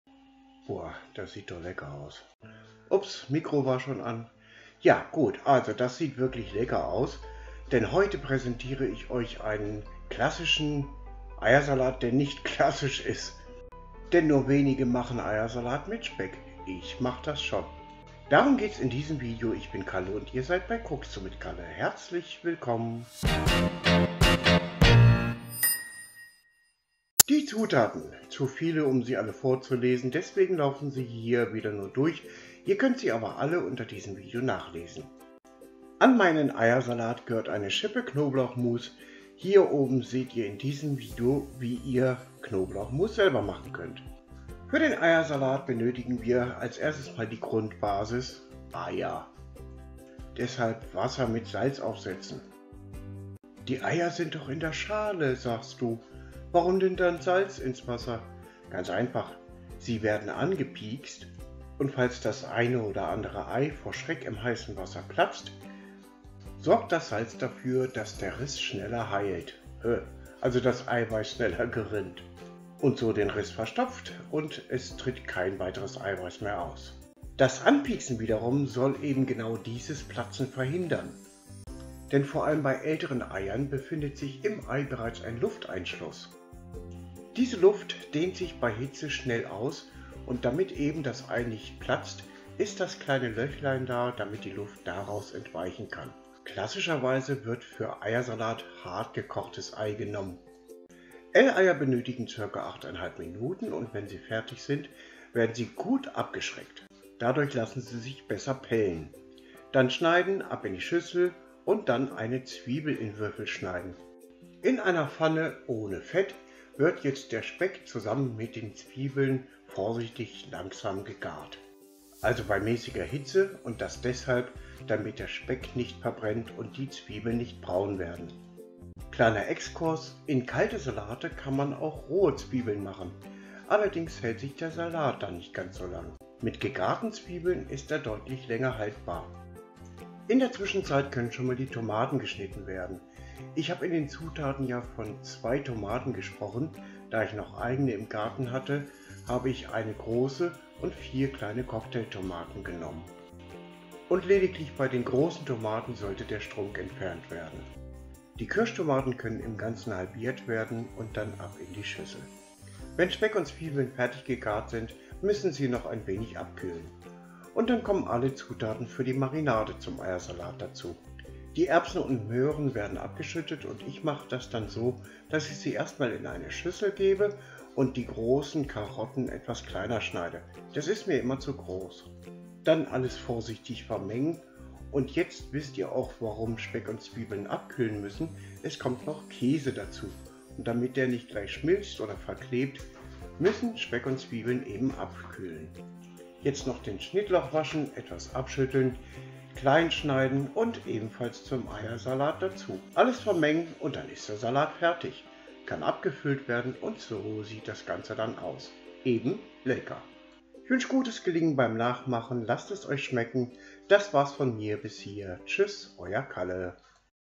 Fetziger Eiersalat mit Speck, saulecker. Let's fetz.
Zutaten:
8 Eier, Größe L
125 g Speckwürfel
1 Zwiebel, mittelgroß
50 g geriebener Käse
1 Dose Erben und Möhren (400 g
Füllmenge, 265 g Abtropfgewicht)
2 Tomaten
¼ Bund Schnittlauch (Zirka-Angabe, da aus
eigenem Kräutergarten)

Für die Marinade:
2 EL Mayonnaise
2 EL Tafelessig
100 g Naturjoghurt, 3,5%
½ TL Knoblauchmus, selbstgemacht (Link zum Video siehe unten)
1 TL Senf

▬ Mein Kanal ▬▬▬▬▬▬▬▬▬▬▬▬

▬ Empfohlene Videos ▬▬▬▬▬▬▬▬▬▬▬▬

▬ Social Media ▬▬▬▬▬▬▬▬▬▬▬▬▬▬▬

►Facebook-Seite:  / Cookst du mit Kalle
►Facebook-Gruppe:  / Kochen, Backen, Kalte Küche